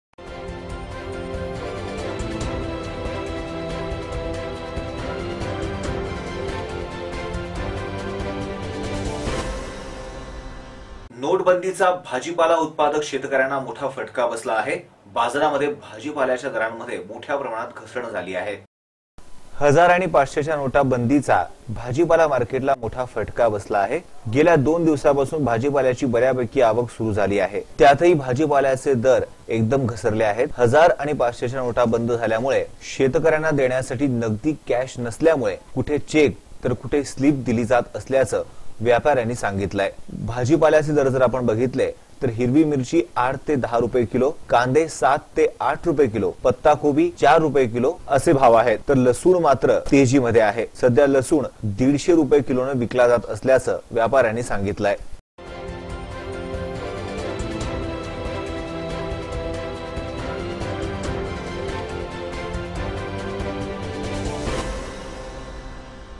औरंगाबाद नोट बंदीचा भाजीपाला उत्पादक शेतकNयांना मोठा फटका बसला बाजारात भाजीपाल्याच्या भावांमध्ये
औरंगाबाद :- नोट बंदीचा भाजीपाला उत्पादक शेतकNयांना मोठा फटका बसला बाजारात  भाजीपाल्याच्या भावांमध्ये मोठ्या प्रमाणात घसरण झाली आहे.